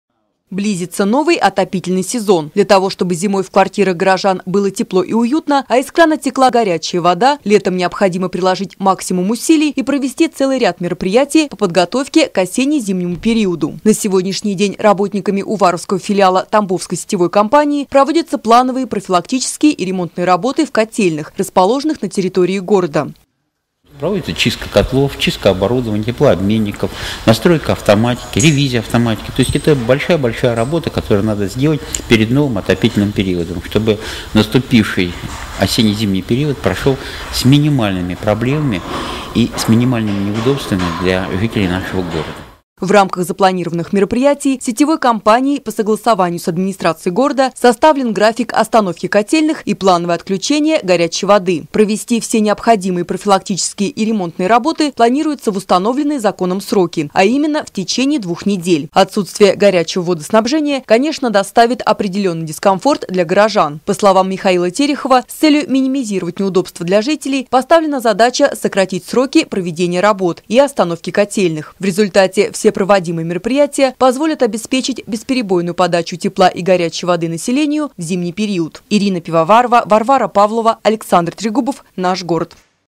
Профилактика работы городских котельных
Город готовится к новому отопительному сезону. В рамках запланированных мероприятий Тамбовской сетевой компанией составлен график остановки котельных для проведения необходимых профилактических работ и ремонта имеющегося оборудования, что создаст для горожан определенные неудобства в виде планового отключение горячей воды.